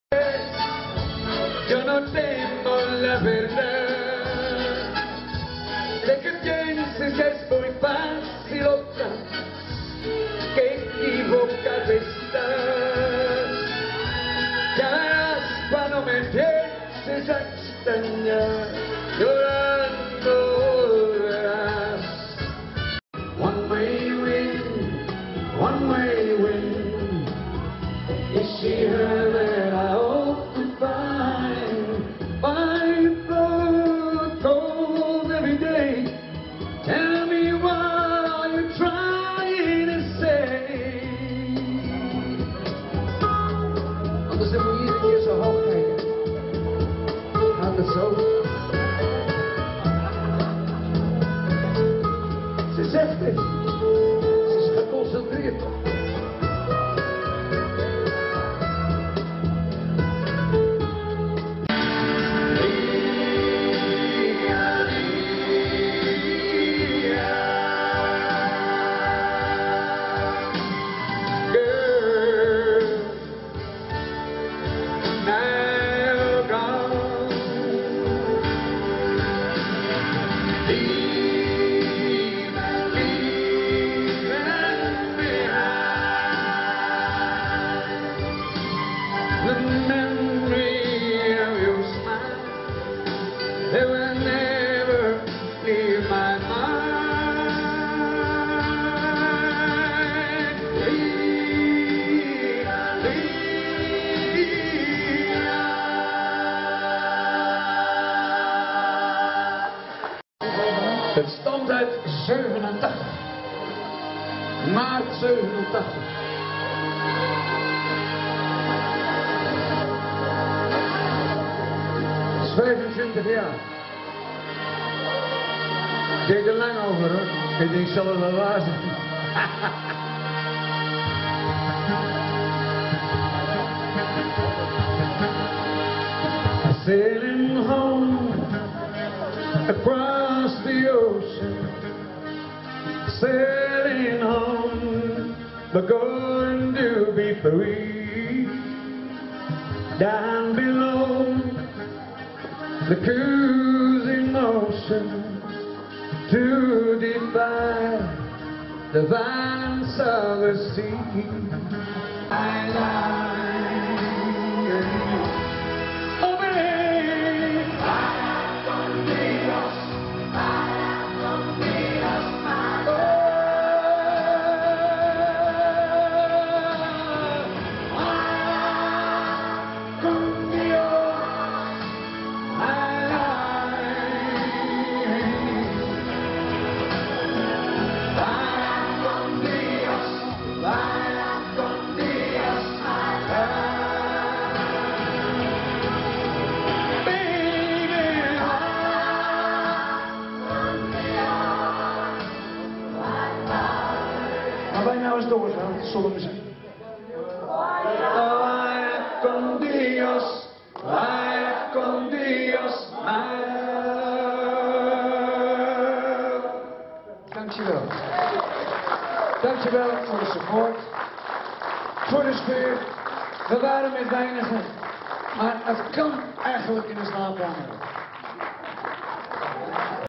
Diner Ansant Piet Veerman 21 april 2012 Eindhoven overzicht deel 3
Van de gehele avond van het Diner Ansant heb ik vele foto's en opnames gemaakt. Met een slightshow deel 3 van vele foto's met de originele sound dat ik ook heb gehoord wil ik even terugblikken wat er op 21 april heeft afgespeeld in Eindhoven.